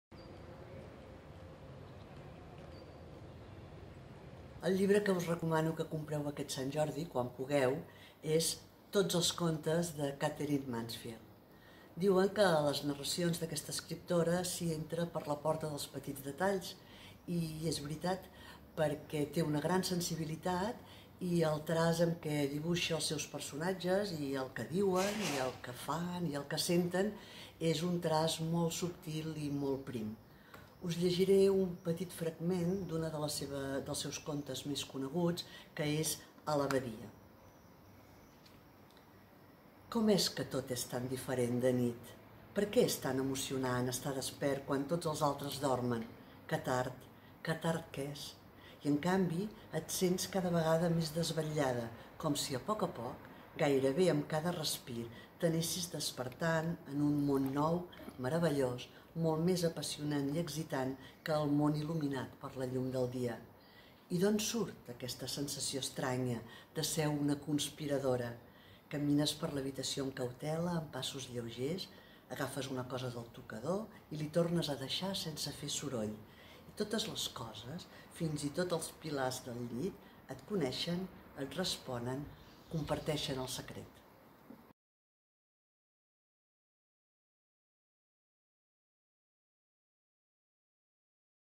SantJordiBCN20 - Maria Mercè Roca
Maria Mercè Roca recomana 'Tots els contes' (Proa, 2018) de Katherine Mansfield

Aquest vídeo s’emmarca dins “Sant Jordi: llibres en xarxa”, una acció virtual en què una cinquantena d’escriptors i escriptores i 10 bibliotecaris de la xarxa de biblioteques públiques de la ciutat recomanaran un llibre, amb l’objectiu que els lectors i lectores el reservin a la seva llibreria de proximitat per tal de recollir-lo quan s’acabi el confinament o bé l’adquireixin durant la jornada de Sant Jordi prevista per a l’estiu.